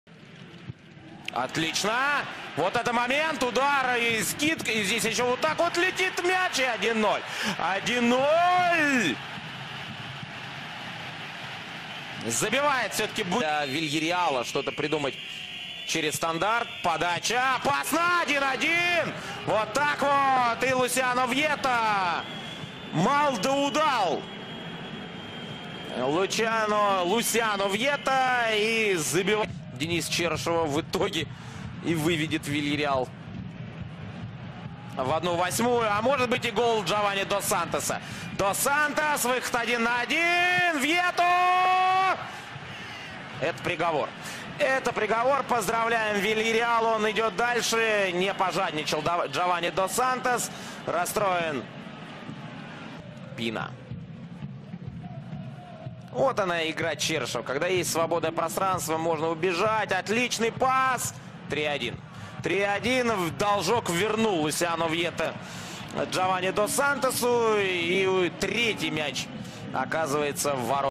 All Goals Salzburg vs Villarreal 1-3 26.02.2015 Europa League
Salzburg Villareal 1-3